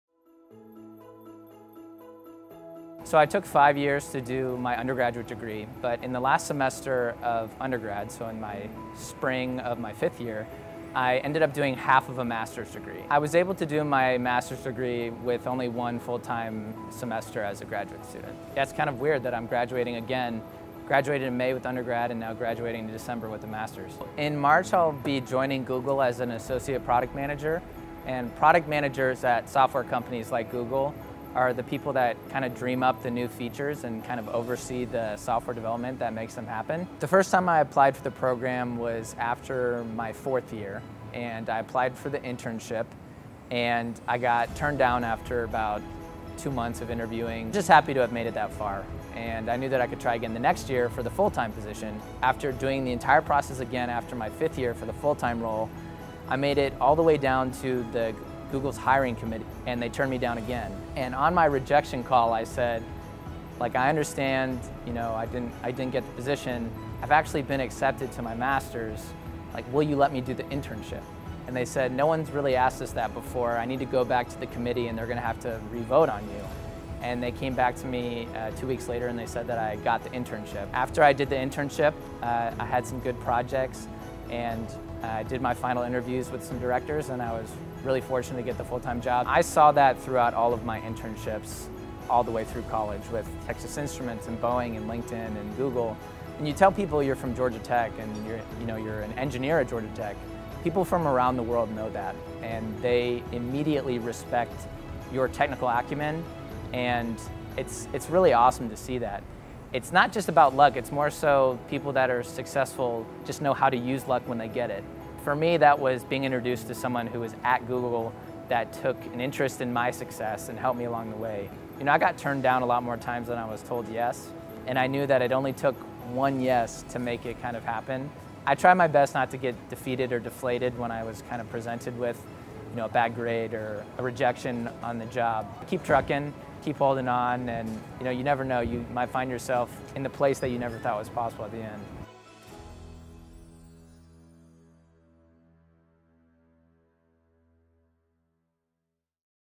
2017 Georgia Tech Graduate: Cole Bevis
After some incredible internships and completing his undergraduate degree Cole Bevis is graduating with a master's in electrical and computer engineering from Georgia Tech and he completed it in just one full time semester. Now he's headed to Google where he's landed his dream job.